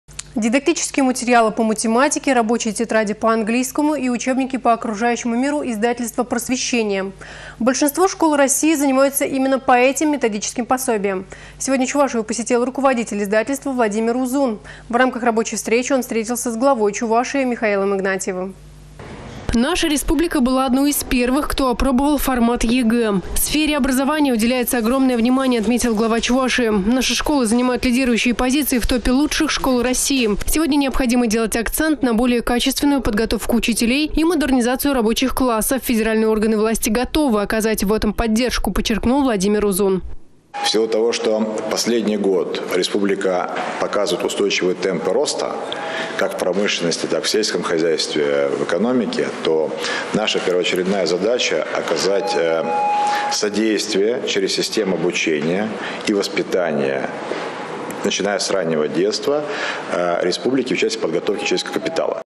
Чувашию посетил руководитель издательства — Владимир Узун.
Дидактические материалы по математике, рабочие тетради по английскому и учебники по окружающему миру издательства «Просвещение». Большинство школ России занимаются именно по этим методическим пособиям. Сегодня Чувашию посетил руководитель издательства — Владимир Узун. В рамках рабочей встречи он встретился с Главой Чувашии — Михаилом Игнатьевым.